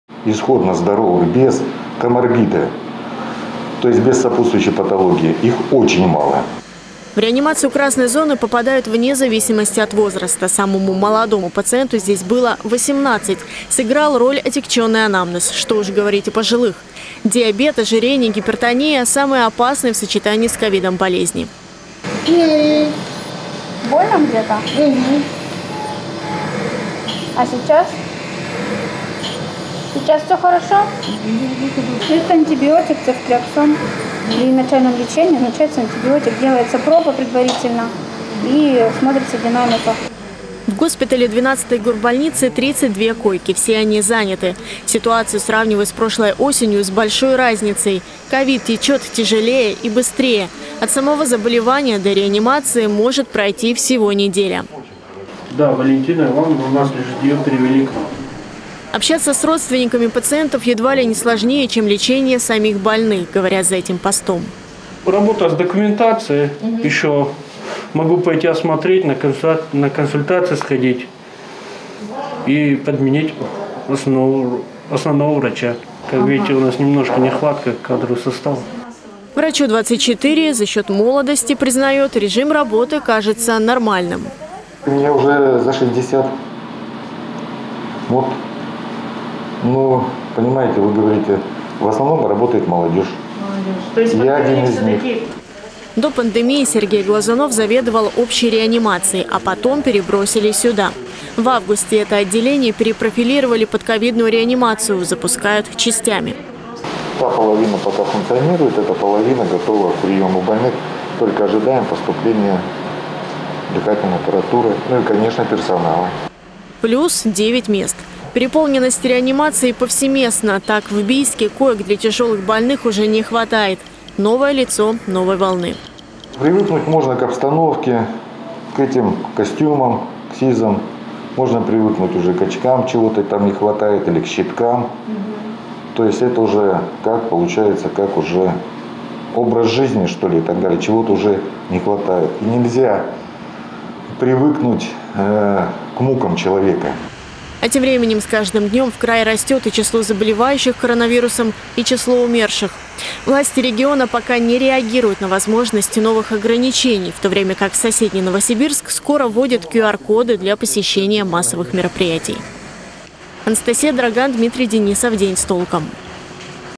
Никто не застрахован: тяжелые отделения красных зон – реанимации – заполнены
Никто не застрахован. В очередную, набирающую обороты волну пандемии врачи не перестают повторять: коронавирус побеждает пациентов вне зависимости от возраста. Самые тяжелые отделения красных зон – реанимации – заполнены. Так, в ковидном госпитале 12-й горбольницы Барнаула готовят к открытию еще одно крыло реанимации. После долгого перерыва мои коллеги вновь отправились в красную зону